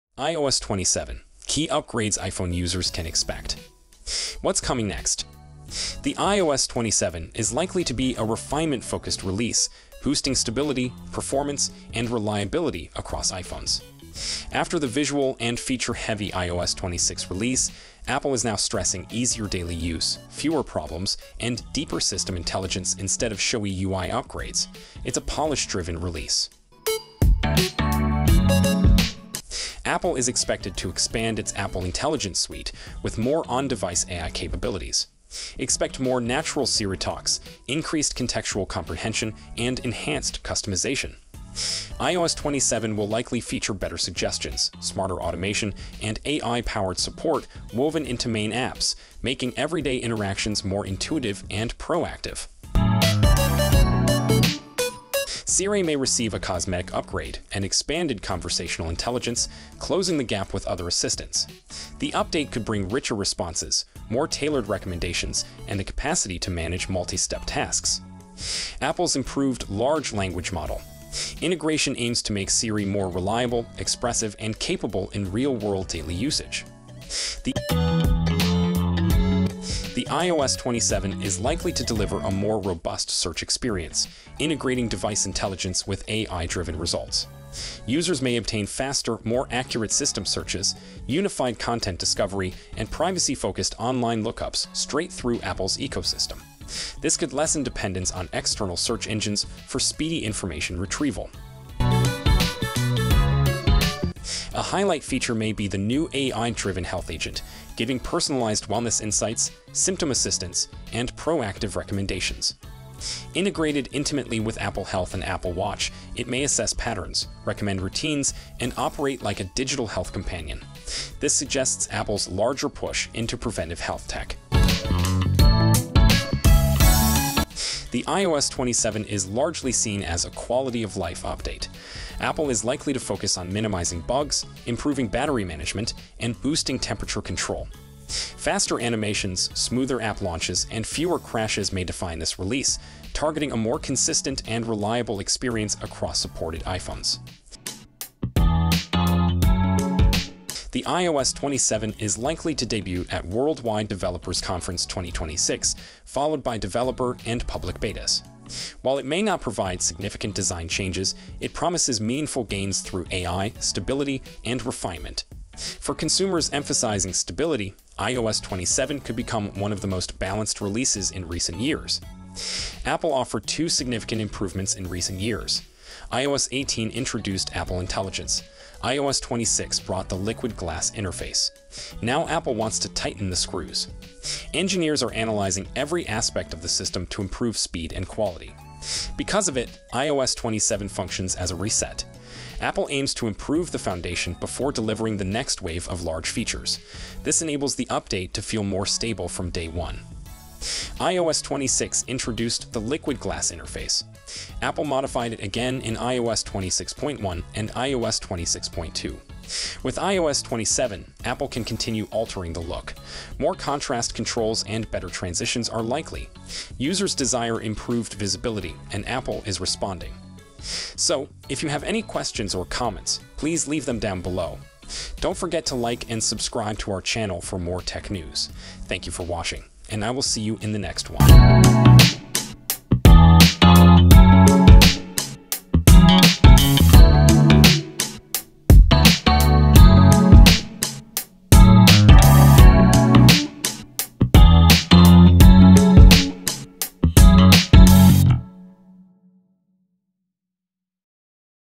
iOS 27 - 6 Mind Blowing Features you need to know!
iOS 27 is shaping up to be a refinement-focused update that prioritizes stability, performance, and system intelligence over flashy new features or major UI changes. After the visually striking and feature-rich iOS 26, Apple aims to make everyday iPhone use smoother and more intuitive with enhanced reliability and fewer bugs. A key highlight of iOS 27 is the expansion of Apple’s on-device AI capabilities, improving Siri’s conversational abilities, automation, and system-wide intelligence. Siri is expected to become more expressive and capable of handling multi-step tasks, thanks to better integration of large language models (LLMs). The update also promises AI-powered search enhancements, delivering faster and more accurate results within Apple’s ecosystem, reducing reliance on external search engines while maintaining user privacy.

Another exciting addition could be the Health Agent, an AI-driven wellness tool integrated with Apple Health and Apple Watch, which offers personalized health insights and proactive recommendations for better preventive care. On the technical side, iOS 27 focuses heavily on performance improvements such as better battery management, faster animations, smoother app launches, and enhanced temperature control, ultimately aiming for a more consistent and stable user experience.

Highlights.
- iOS 27 focuses on stability, performance, and reliability rather than major UI changes.
- Expanded on-device AI capabilities will improve Siri’s conversational skills and automation.
- Siri’s upgrade includes richer responses and multi-step task management using enhanced LLM integration.
- AI-powered search will provide faster, more accurate, and privacy-focused results within Apple’s ecosystem.
- Introduction of an AI-driven Health Agent offering personalized wellness insights and recommendations.
- Improvements in battery management, app launch speed, animations, and temperature control.
- Expected release at WWDC 2026, with subtle UI refinements to improve contrast and visibility.

Keywords.
- iOS 27
- Apple Intelligence
- Siri upgrade
- AI-powered search
- Health Agent
- Performance improvements
- WWDC 2026.